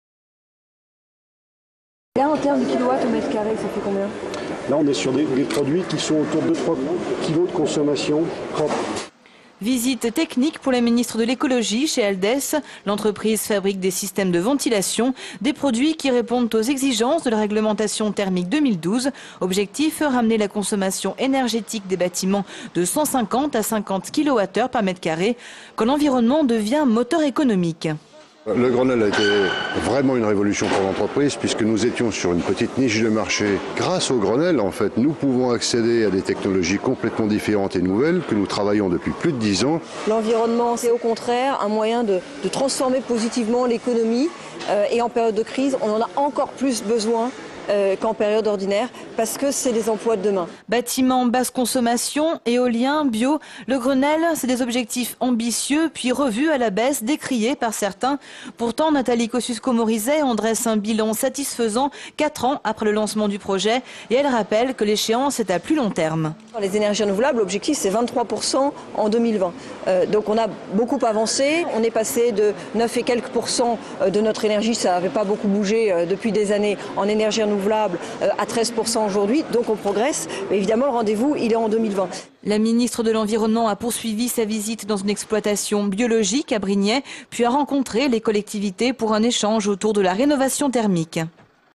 Nathalie Kosciusko-Morizet en visite à Lyon
Sites webs/blogs, copiez cette vidéo avec le bouton "Intégrer" !
 La ministre de l'écologie Nathalie Kosciusko-Morizet est en visite à Lyon à l'occasion des 4 ans du Grenelle de l'Environnement.